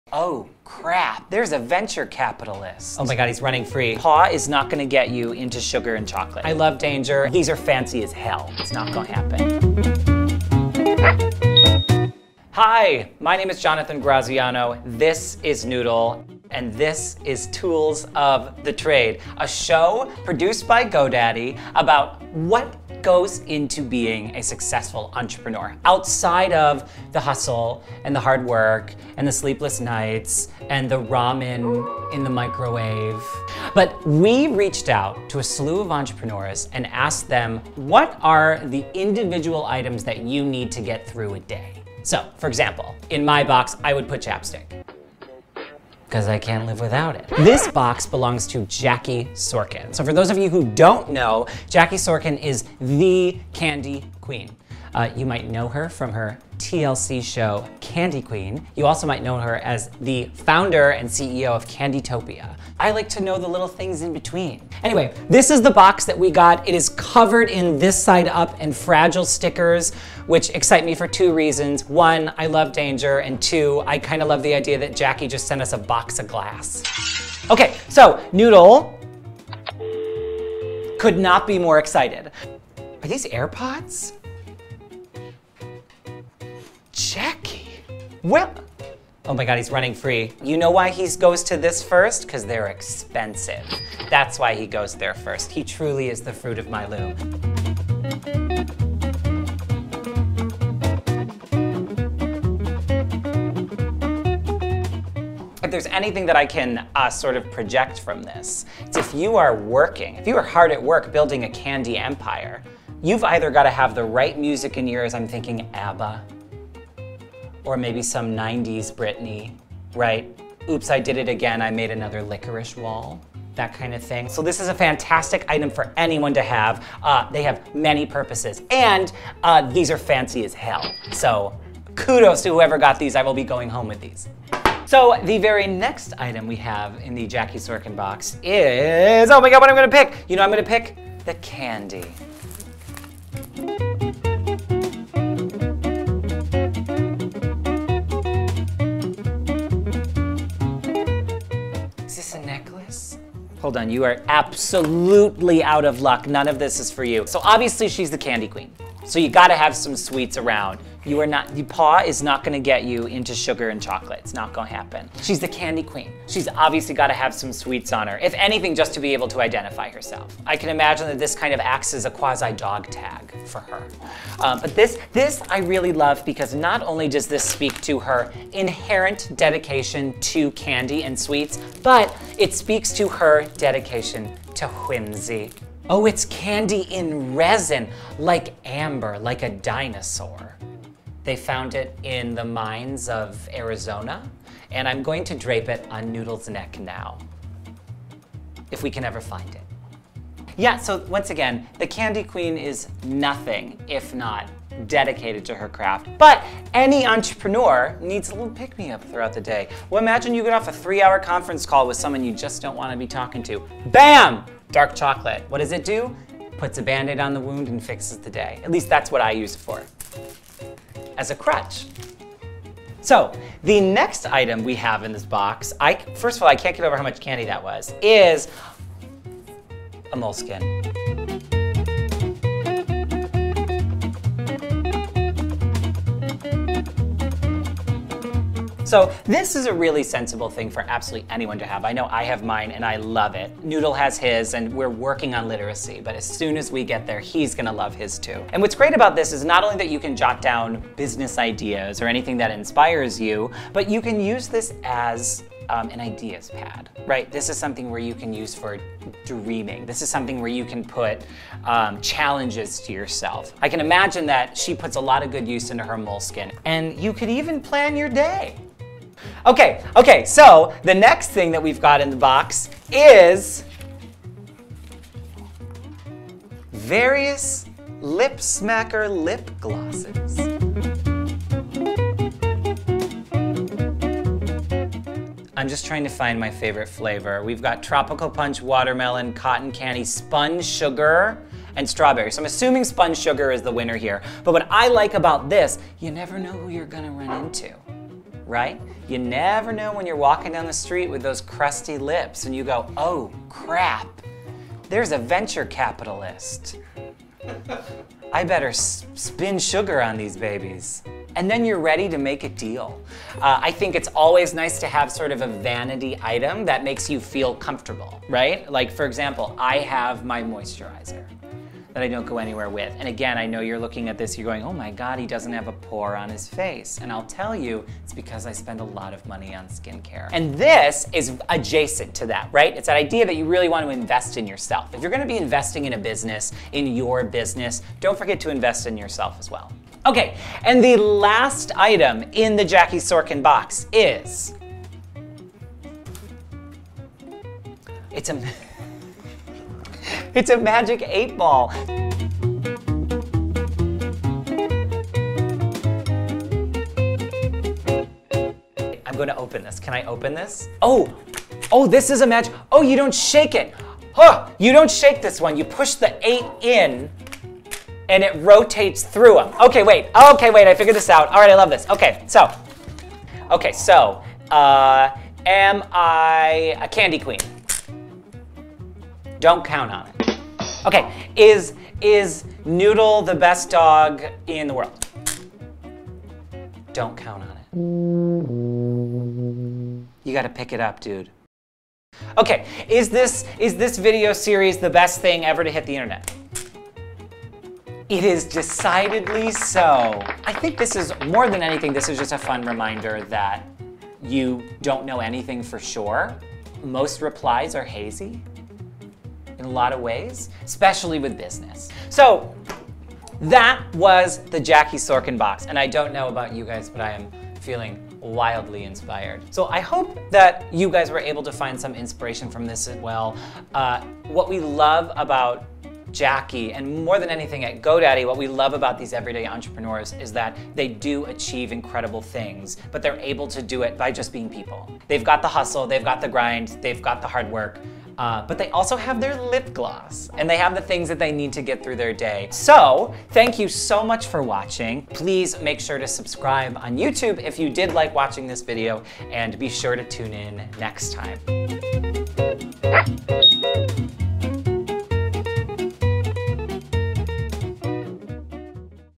Unboxing the Candy Queen | Jackie Sorkin
In episode 1 of Tools of The Trade, host Jonathan Graziano and his pug Noodle unbox "The Candy Queen” Jackie Sorkin’s everyday necessities.

Known for her TLC show Candy Queen, Jackie Sorkin is also the CEO of Hollywood Candy Girls Inc., and co-founder/co-creator of Candytopia, a pop-up mini theme park made entirely of candy. When you’re ruling the candy world, you need creative (and tasty) ways to navigate the sugar-filled highs and lows of entrepreneur life. Jackie gives us a peek into her typical day and shares the goods she keeps on hand to manage the sweetest gig in town.